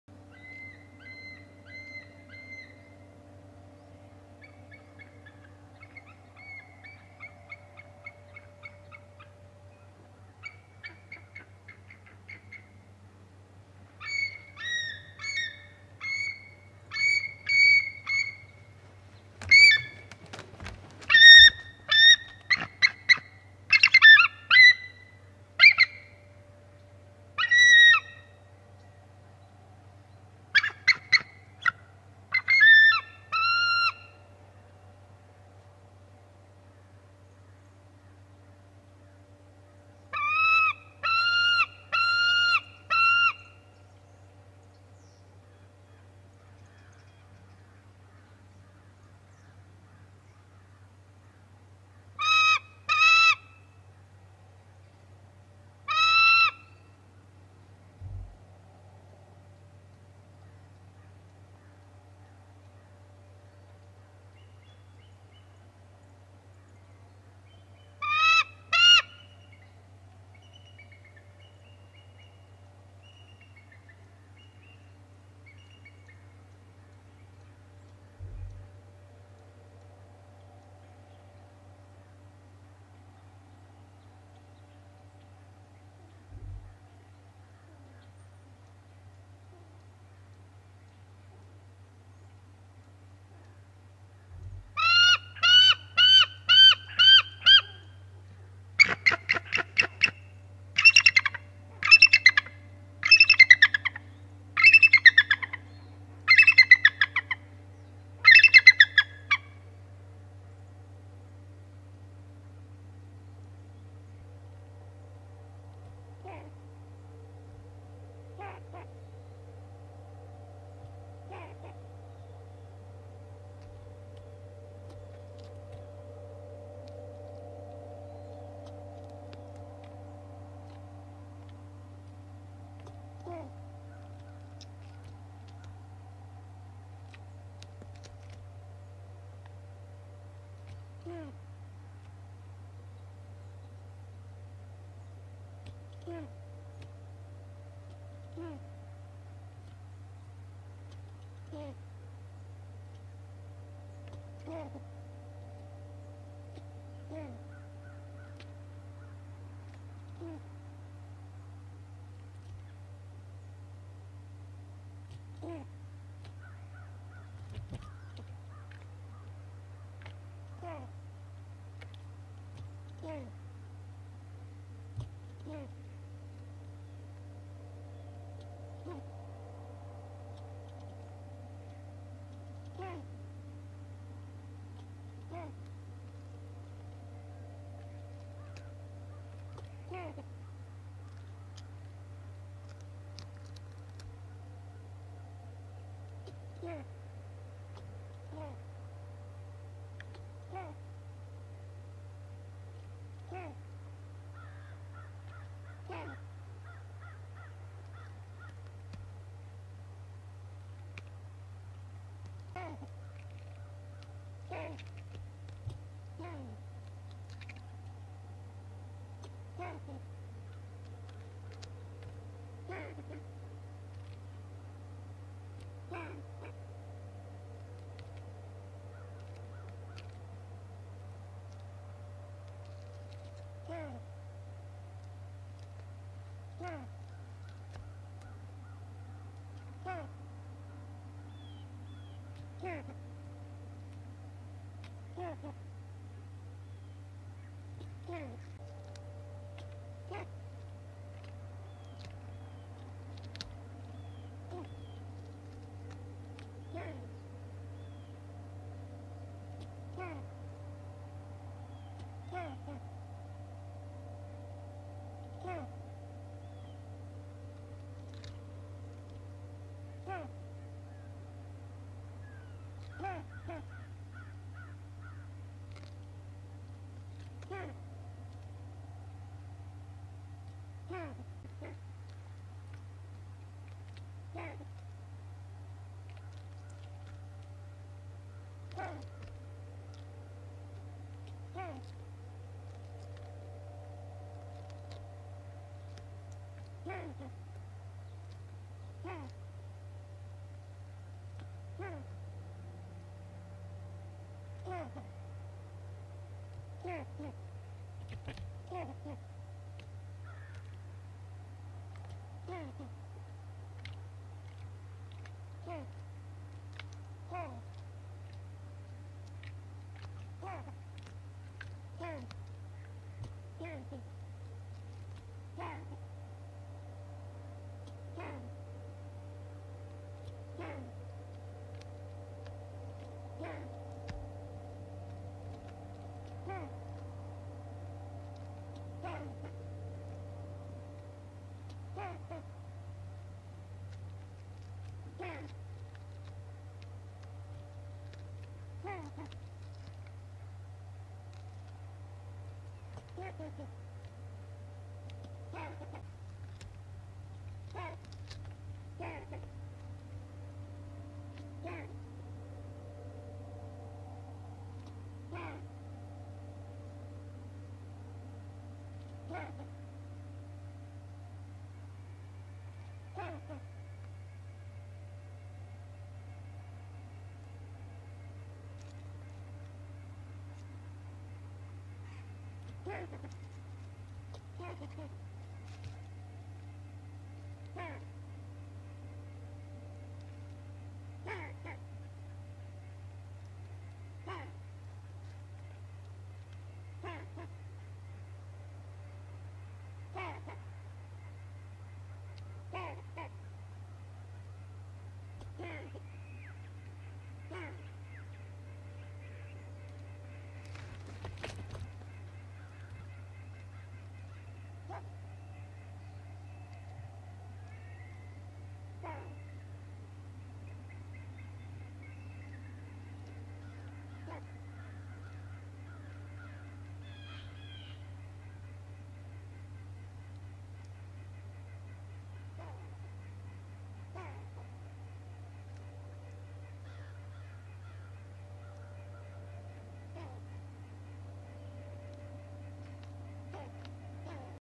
Decorah North Eagles BreakFish & Brave Squirrel 11/12/17
Sunday AM edited clips, about 7:30am CT. Looks like Mom North flies in with fish, and some eagle chatter. While MomN eats her fish a, brave squirrel watches her and chatters lol.There is also adult eagle that flies from left 2 the right, and a sub adult flies
from the right 2 the left.